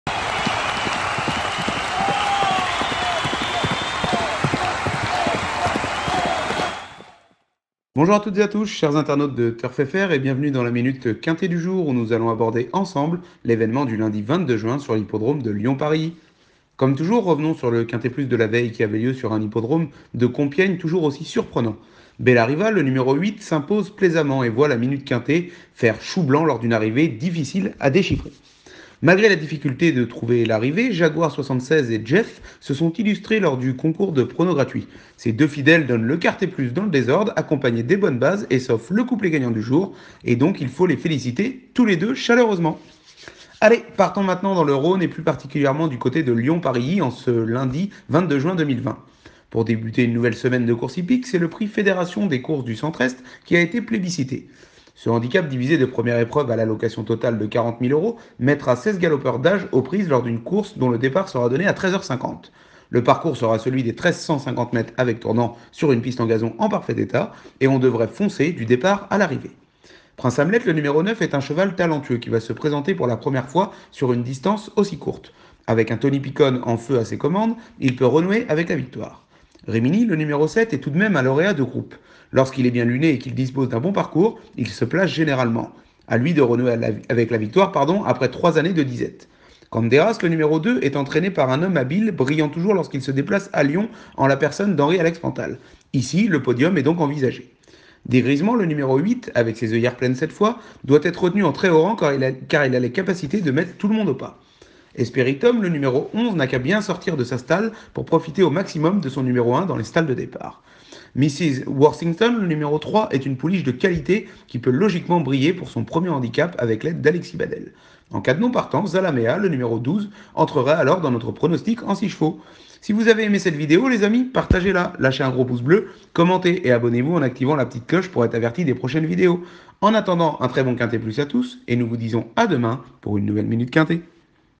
Minute Quinté TURF FR : PRIX FEDERATION DES COURSES DU CENTRE EST - Lundi 22 Juin - Lyon PMU #206529
🥇 Retrouvez le pronostic exclusif de notre expert Turf Lucius Simenon pour le PRIX FEDERATION DES COURSES DU CENTRE-EST Lundi 22 Juin 2020 en France à l'hippodrome de Lyon Parilly en France.

✅ NOS LOGICIELS TURF :
✔️ Logiciel TURF INFO Professionnel Edition:

Bientot 3000 abonnés sur notre chaine Youtube, un grand merci les amis !